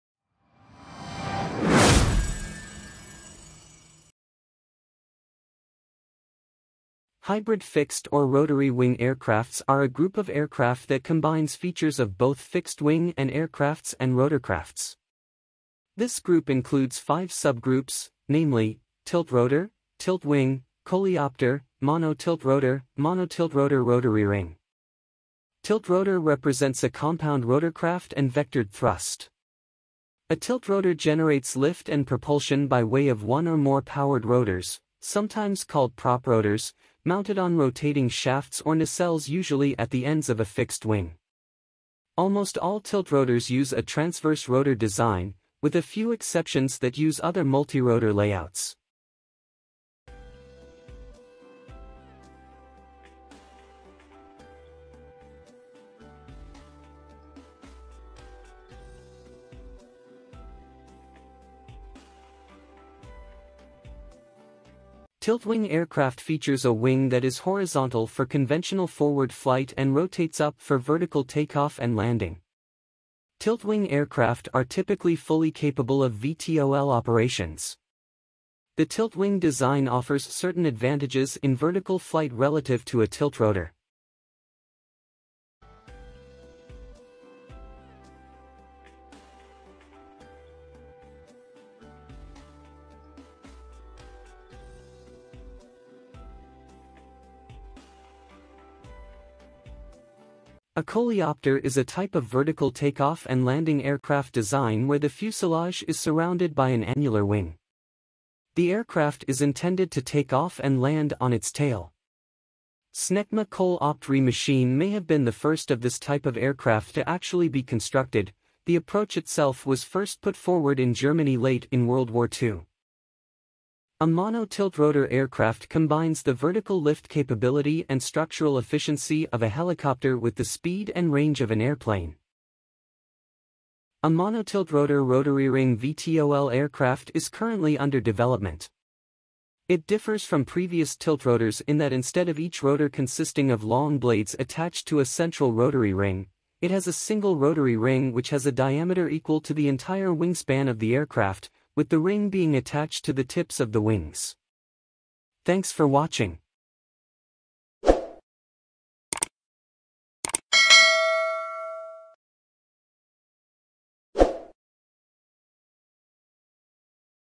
Hybrid Fixed/Rotary Wing Aircrafts | Aircraft Classification
In this video, we explore the cutting-edge world of hybrid fixed/rotary wing aircraft—an exciting fusion of traditional airplane and helicopter technologies. These innovative aircraft combine the efficiency of fixed wings with the maneuverability of rotary wings, offering incredible versatility in both civilian and military applications.

Discover:
What Makes Them Hybrid? – Understanding the blend of fixed and rotary wing systems.
Tiltrotors & Other Designs – Learn about tiltrotor aircraft like the V-22 Osprey and other unique designs.
Advantages & Use Cases – How hybrid aircraft excel in vertical takeoffs, hovering, and long-range flight.
Future of Aviation – Explore how these aircraft could revolutionize transport, search and rescue, and military operations.

By the end of this video, you’ll gain insight into the groundbreaking technologies shaping the future of aviation!